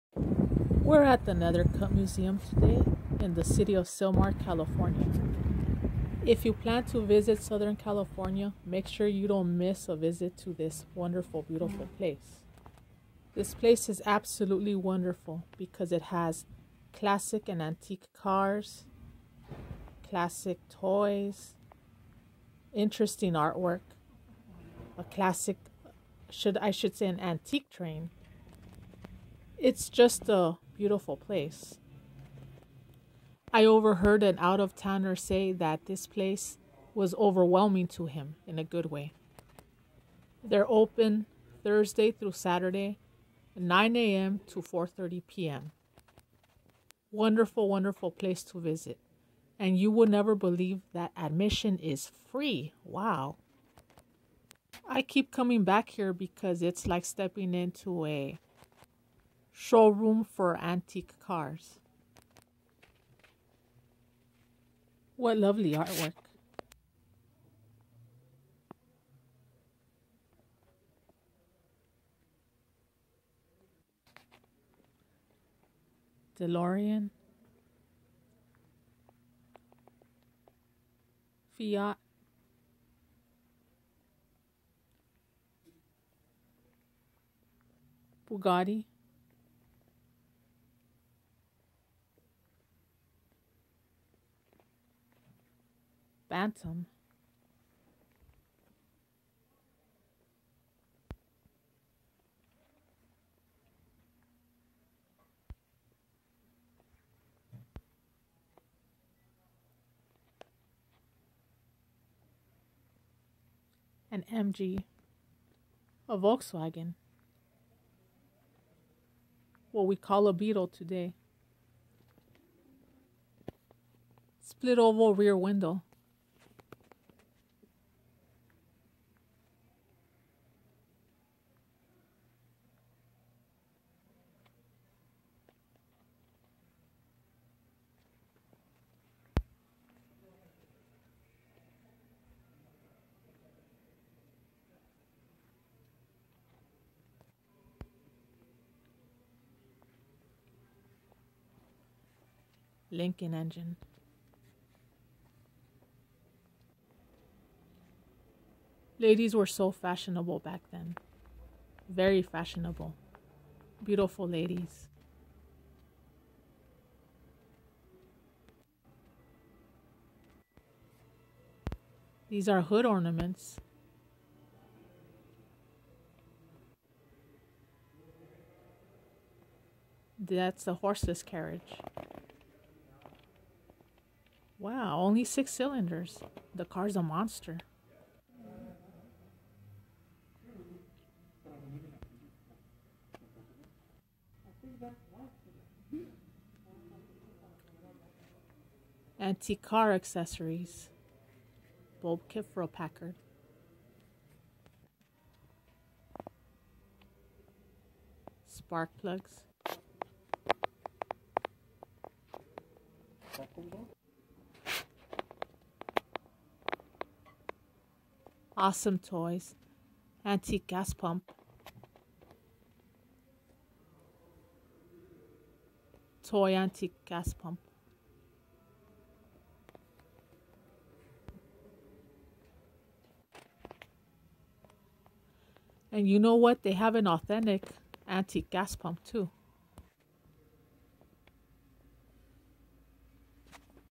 Visit an ANTIQUE CAR SHOWROOM!
This Antique Car Showroom is The Nethercutt Museum, located in Sylmar, CA. This museum as of Feb 17, 2022 is open from Thursday to Saturday, 9am to 4:30pm. It houses part of the Classic and Antique Car Collection of the late Mr and Mrs Nethercutt, owners of Merle Norman Cosmetics. Admission is currently Free. If you love Antiques and Classics, or you just want something to do, or you have out-of-town guests and you need to entertain them, don't miss the opportunity to visit this beautiful, once-in-a-lifetime place!